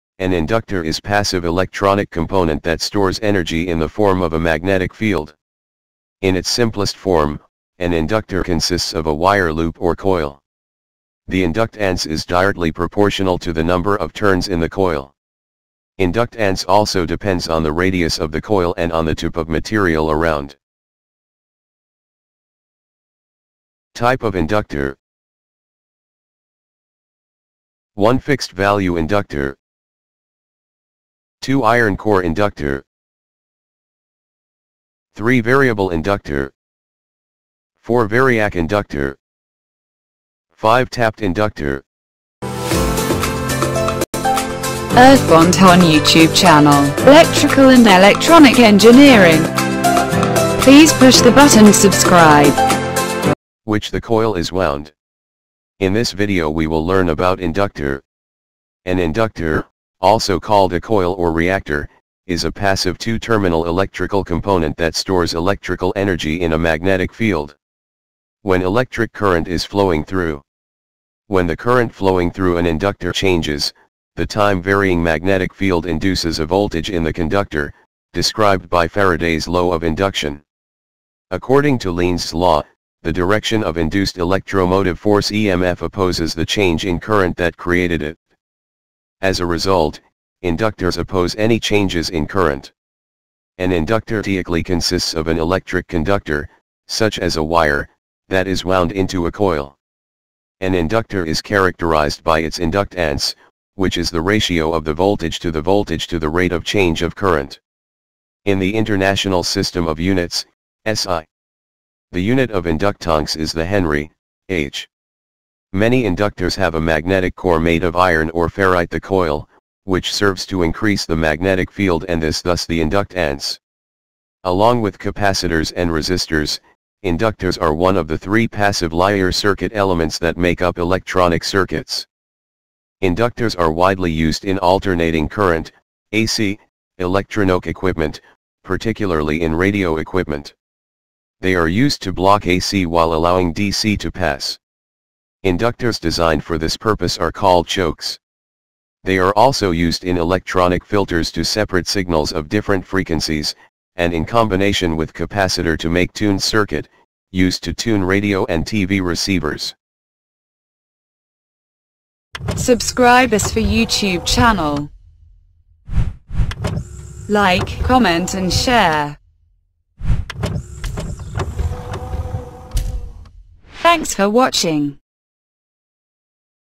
inductor Basics | Electronics Basics | Earth Bondhon
The basics of how inductors work, a demo showing an inductor filtering out high-frequency signals,  An inductor is a passive electronic component that is capable of storing electrical energy in the form of magnetic energy. Basically, it uses a conductor that is wound into a coil, and when electricity flows into the coil from the left to the right, this will generate a magnetic field in the clockwise direction.